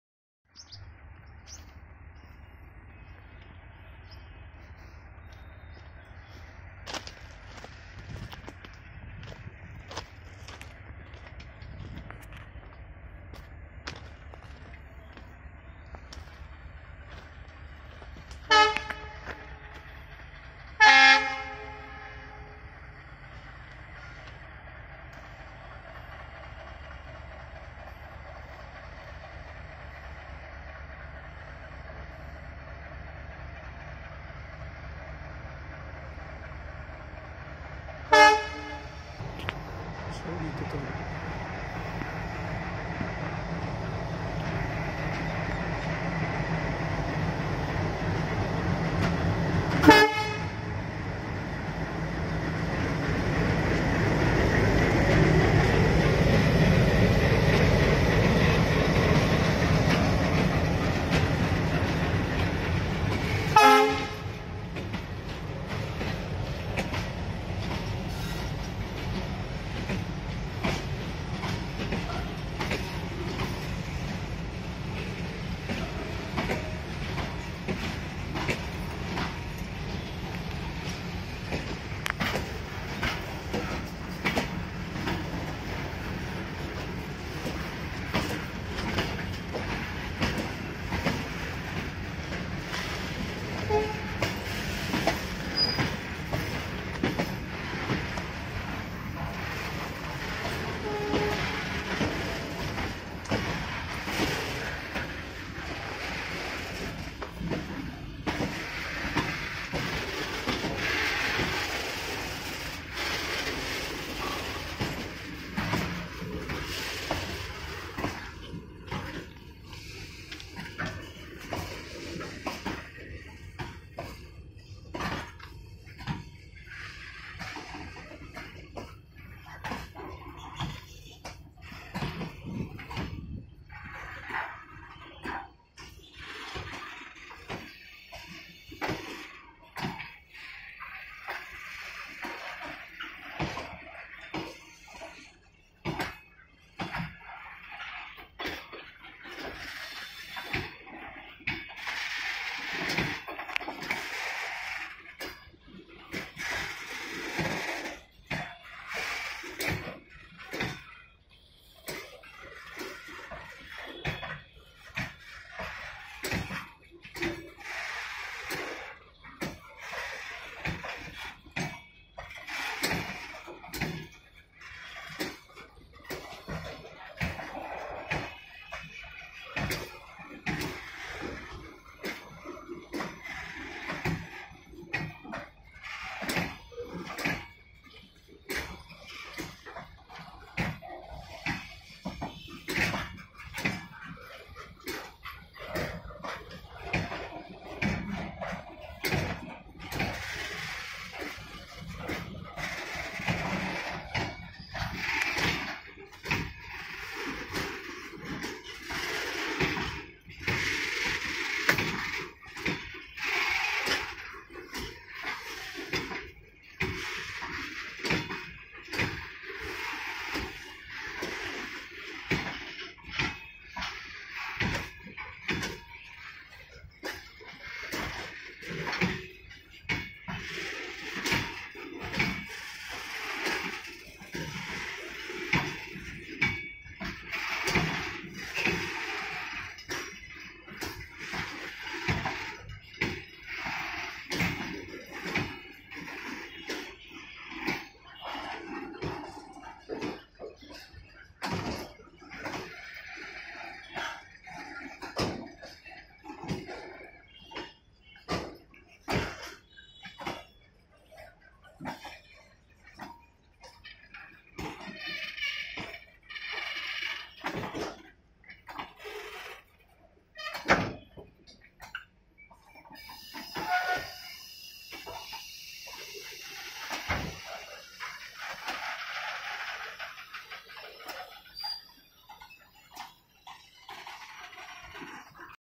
Loaded INDIAN BCNA Train in Bangladesh with WDM-3D 6536 | Ishwardi Junction Yard
Indian locomotive class WDM-3D

The Indian locomotive class WDM-3D is a class of diesel-electric locomotive that was developed in 2003 by Banaras Locomotive Works (BLW), Varanasi for Indian Railways. The model name stands for broad gauge (W), Diesel (D), Mixed traffic (M) engine with 3300 horsepower (3D). The engine is classified WDM-3D though it outputs only 3300 hp and not 3400 hp as the name should suggest. They entered service in 2003. A total of 590+ WDM-3D were built at Banaras Locomotive Works (BLW), Varanasi between 2003 and 2016.

The WDM-3D is one of the most successful locomotives of Indian Railways serving both passenger and freight trains. A few WDM-3D units were exported or gifted to neighboring countries like Sri Lanka and Bangladesh. A significant number of these locomotives are still in use, both on mainline and departmental duties. As of January 2020, most of the locomotives still retain "operational status" on the mainline as WDM-3D, with further examples having been converted from WDM-3B.

It is the higher-powered version of the Indian locomotive class WDM-3A. These locos have a 3300 hp power pack, with maximum available traction power of 2925 hp. The engine is an enhanced version of the 16-cylinder Alco 251C model with a max. speed of 160 km/h. The bogies are Improved Fabricated (welded) Alco High-Adhesion Co-Co bogies with stem type vertical and lateral dampers in place of 'eye' type which helps reduce wheel slip and maintenance.

The loco features left hand drive, WDG-3A style High Adhesion bogies, air cylinder under foot-board, WDP-4 style fuel tanks, engine doors like WDP-4, marker lights outside cabin doors, electronic horn, High capacity buffers. Components and auxiliaries have been improved with the aim of making the duty schedule longer between maintenance visits to the shed.

On 27 July 2020, due to overaging of existing Bangladesh Railways locomotives, IR gave 10 WDM-3D units for free under its “grant assistance” plan. The Railways has modified the locos to suit the maximum height restrictions in BR and the donated locomotives (6-8 year old) have 28 years of serviceable life left. India formally handed over the locomotives in a virtual ceremony on July 27. The vehicles cost an estimated ₹600m ($US 8m) to manufacture.

January 2022